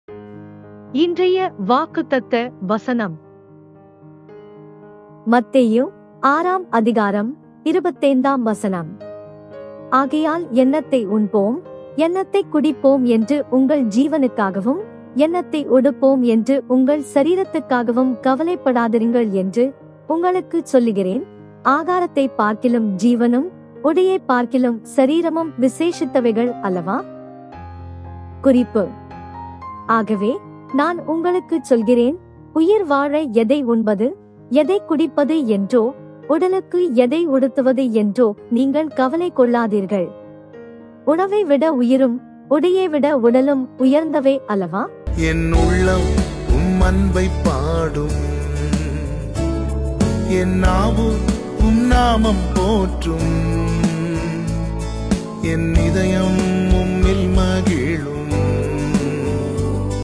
Daily Bible Verse | தினசரி பைபிள் வசனம் | Today Bible Verse in Tamil
ஆகையால் என்னத்தை உண்போம், என்னத்தைக் குடிப்போம் என்று உங்கள் ஜீவனுக்காகவும்; என்னத்தை உடுப்போம் என்று உங்கள் சரீரத்துக்காகவும் கவலைப்படாதிருங்கள் என்று, உங்களுக்குச் சொல்லுகிறேன்; ஆகாரத்தைப் பார்க்கிலும் ஜீவனும், உடையைப்பார்க்கிலும் சரீரமும் விசேஷித்தவைகள் அல்லவா?
[மத்தேயு 6:25]